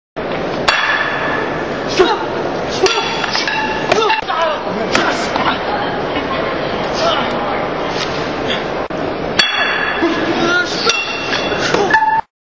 ROS POLICE DEMONSTRATION CAPTAIN LEVINET
Real Operational Sytem by his founder,Captain LEVINET, CN.10e Dan ROS,during the internation event "MILIPOL" in Paris